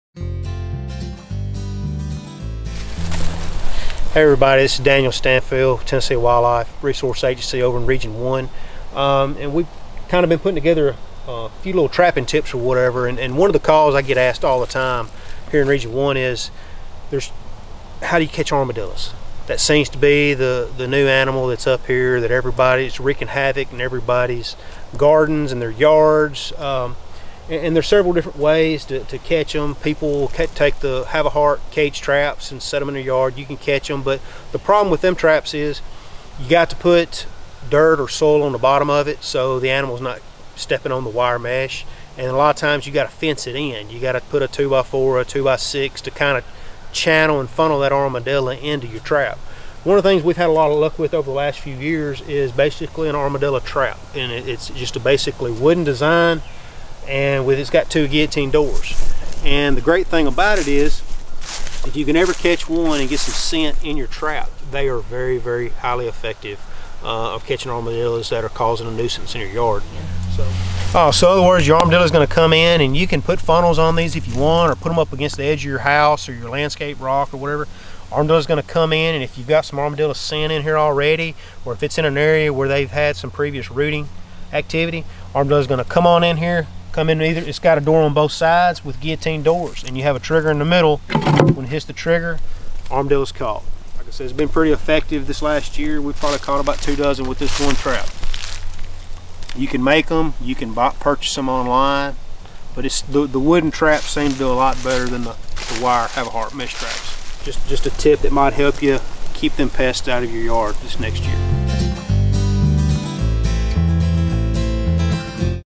Armadillos - TWRA Trapper Tips
Do you have armadillos destroying your yard and want to get rid of them? If so, check out today's Tennessee Trapper Tip, Trapping Armadillos.